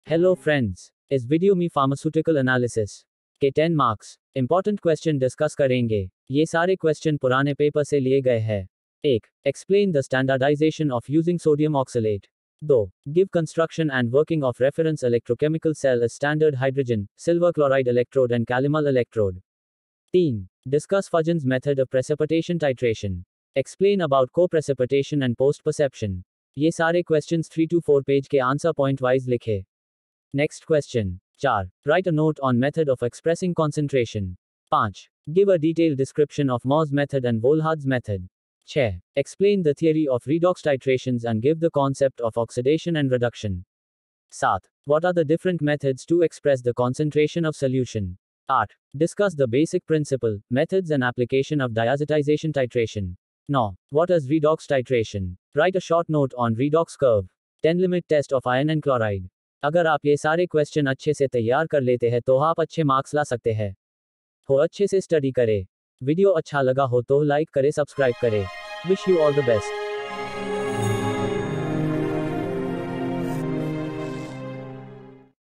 Pharmaceutical analysis||10 marks most important Question's || B.pharma 1st semester exams
pharmaceutical analysis
10 marks most important Question's || B.pharma 1st semester

Idhr udhr na bhatke  ye sare questions ache se solve kr ke study kro ek bhi back nahi ayegi 100%
For  exam

Pharmaceutical microbiology